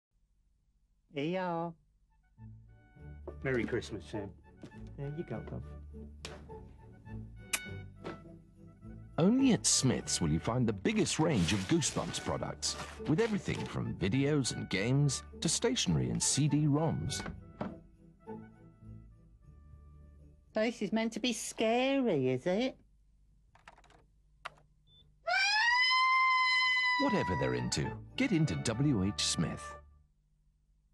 You Plonker, Rodney
Disturbing 1998 advert for WH Smith.

There isn't much left to surprise me these days, but if there's one thing I never thought I'd see, it's the terrifying sight of Rodney Trotter dressed up as a teenage girl - or a teenage boy, or a housewife, for that matter. And, now that I have seen it, I can confirm that my sanity has now been revoked.

What the hell was going on here? Is this how we're meant to celebrate Christmas? Just seeing this imagery is bad enough, but it gets worse. Not only does this "teenage girl" evoke feelings of sheer terror, "she" also manages to destroy the Goosebumps franchise as well. You'll never look at R.L. Stine's fun, creepy books the same way again after you've seen this kook scream like a banshee.

Oh, wow, this is bad. I own everything shown in this advert, but I'm just going to have to burn it all to get the memories of this out of my head. It's going to haunt me for years to come. I'll need therapy. I'll need the sweet release of death. It won't be the turkey going in the oven this Christmas - it'll be me. Thanks, WH Smith.